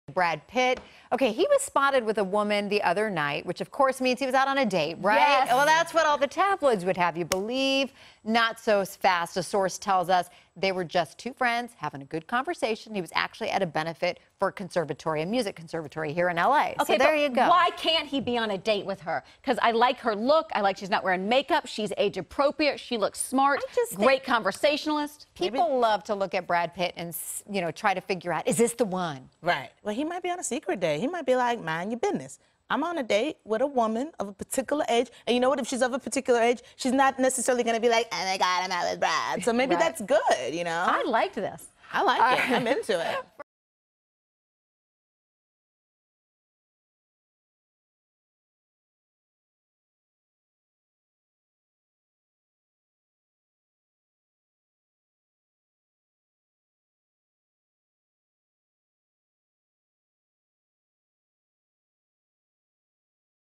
Brad Pitt Does Not Have A New Girlfriend | Access
Brad Pitt was spotted out with a lady friend at dinner, but she's not his girlfriend despite some online rumors. Watch to find out more!